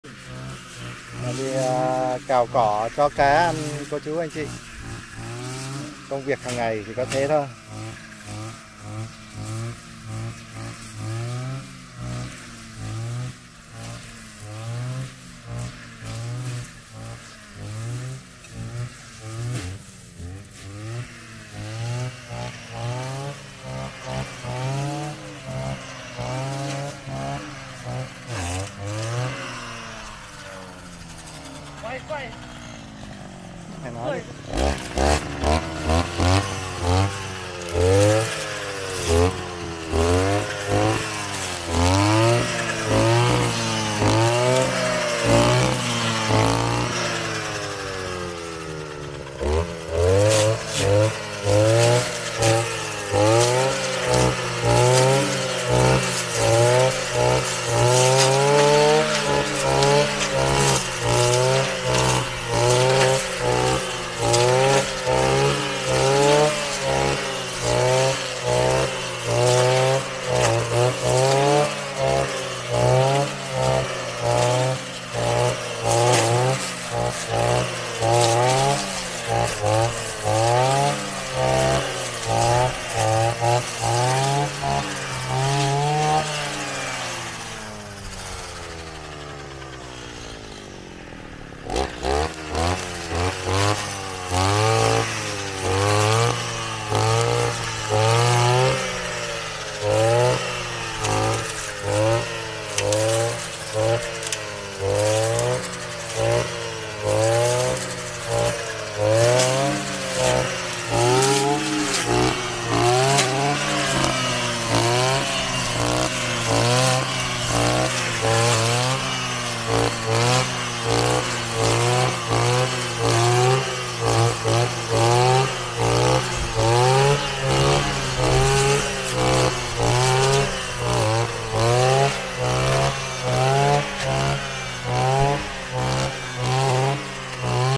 PHÁT CỎ CHO CÁ ĂN QUAY VIDEO CŨNG NGẠI VÌ EM ĐẸP T|Đình Hoàng TV
-Cuộc Sống Tu- PHÁT CỎ CHO CÁ ĂN QUAY VIDEO CŨNG NGẠI VÌ EM ĐẸP TRAI